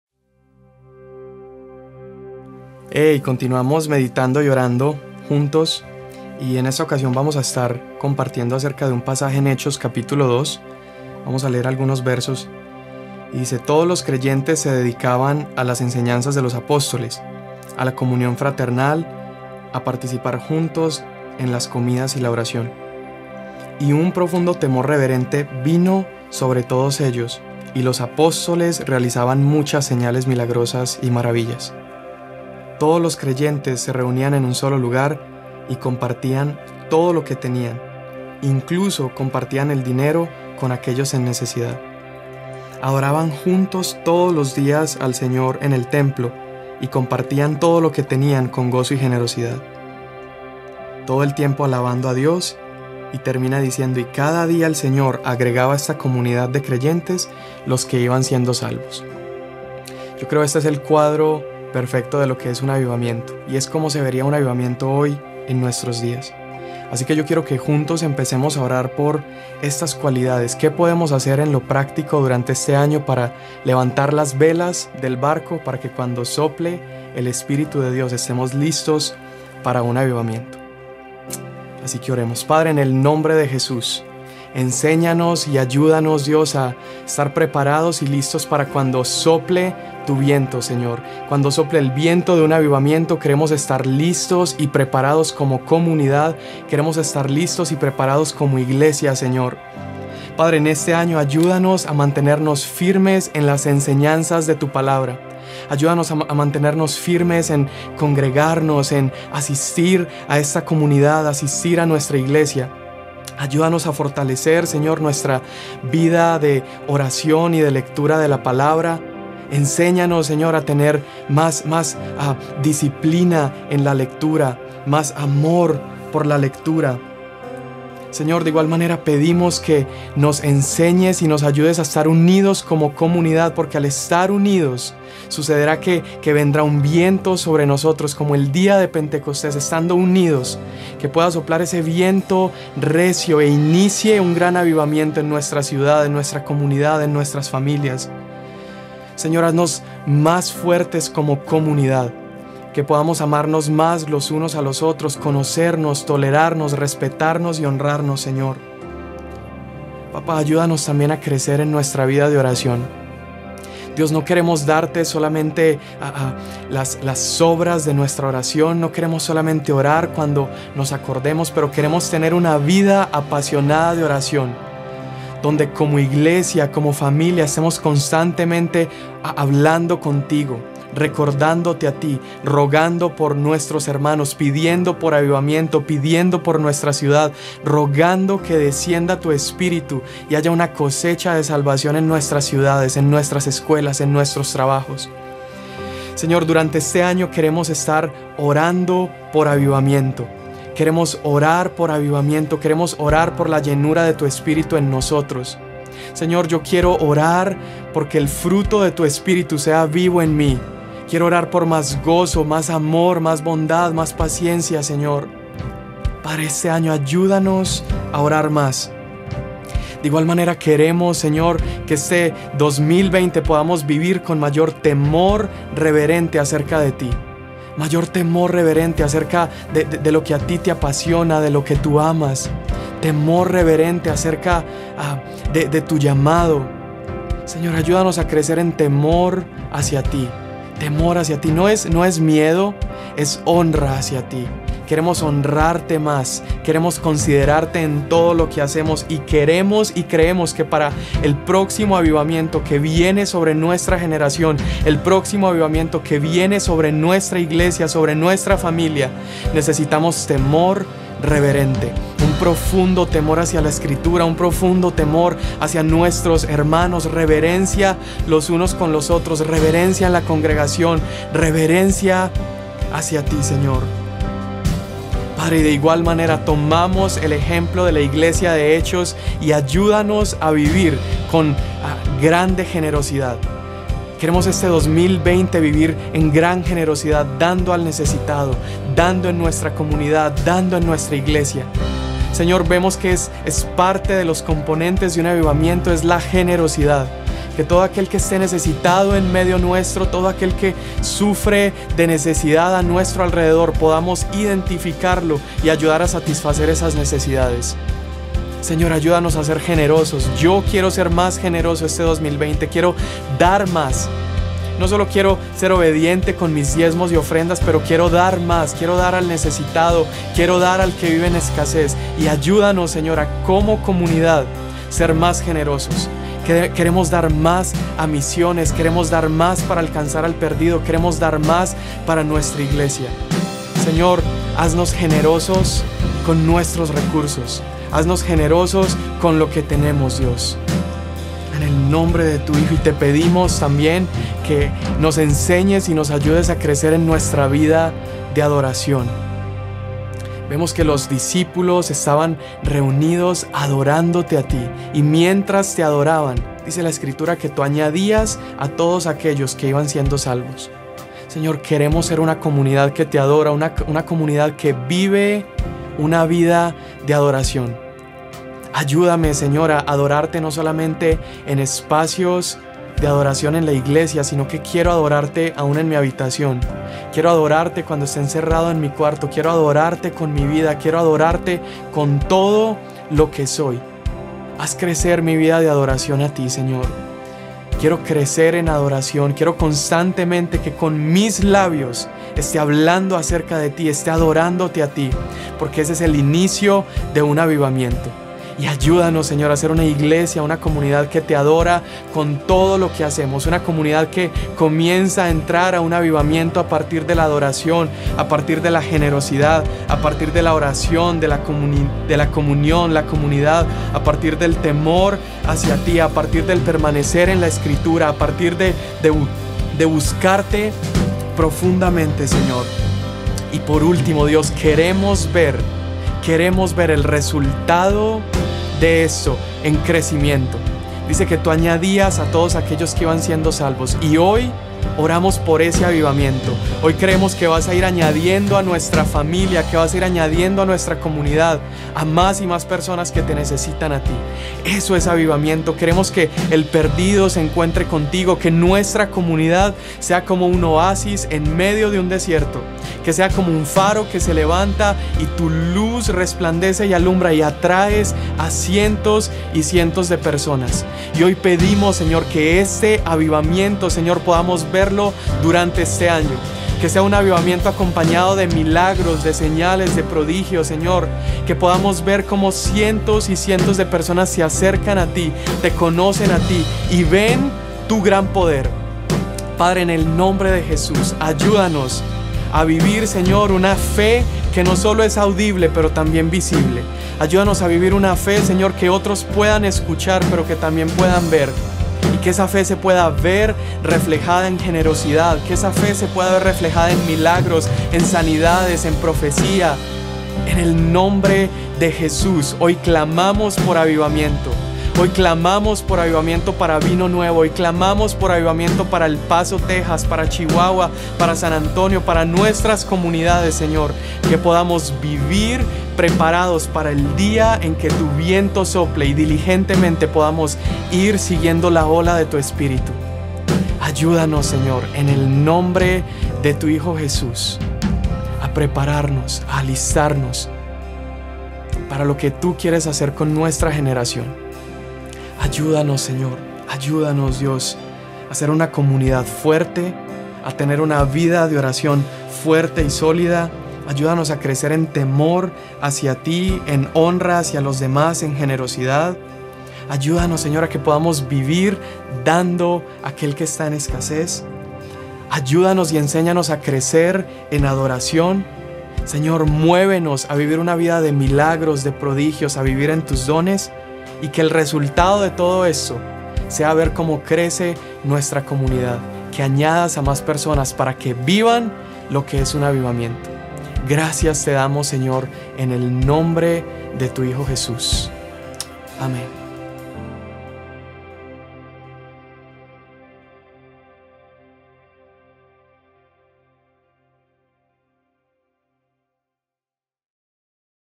30 DÍAS DE ORACIÓN: DÍA 2 - LAS VELAS DEL BARCO
30 DÍAS DE ORACIÓN: DÍA 2 - LAS VELAS DEL BARCO
DÍA 2: LAS VELAS DEL BARCO.

Queremos iniciar el año apartando 10 minutos de nuestro tiempo al día para orar y disfrutar un tiempo con Dios. Ponte cómodo y síguenos durante estos próximos 30 días, estamos seguros de que Dios hablará a tu corazón.